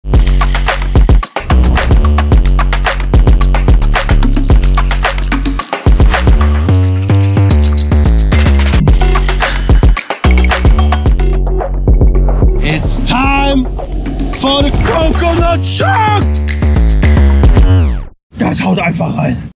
KNOSSI Alge Coconut Shock | LLV Film
Entdecke unser neues Video über den Coconut Shock Likör von Alge - ein sahniger und cremiger Genuss! Möglicherweise bist du schon durch @Knossi  darauf aufmerksam geworden, der diesen Likör liebt und ihn oft in seinen Streams vorstellt. Wie gefällt dir unser kurzer Clip?

Schau gerne auch auf unserer Website vorbei!

ALGE Coconut Shock haben wir selbst erworben.